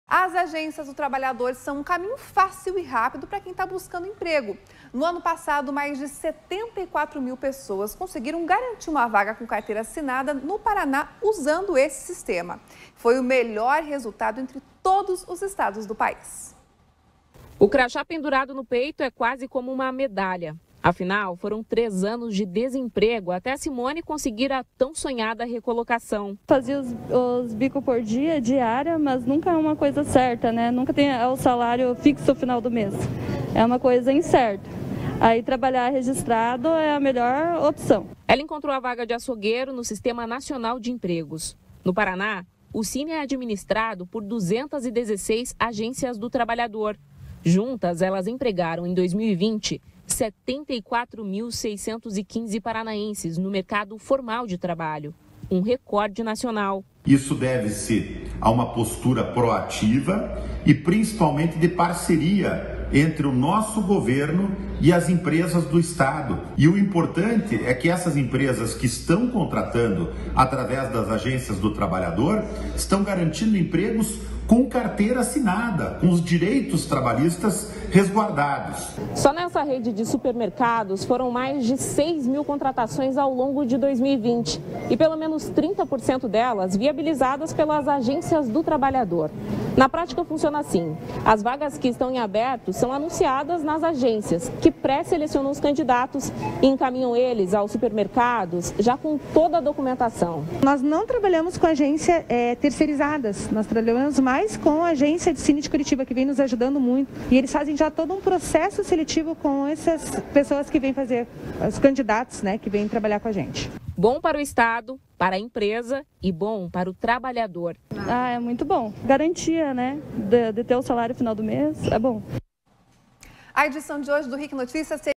Agências do trabalhador no Paraná
As agências do trabalhador são um caminho fácil e rápido para quem está em busca de emprego. No ano passado, mais de 74 mil pessoas conseguiram garantir uma vaga com carteira assinada no Paraná usando esse sistema. foi o melhor resultado entre todos os estados brasileiros.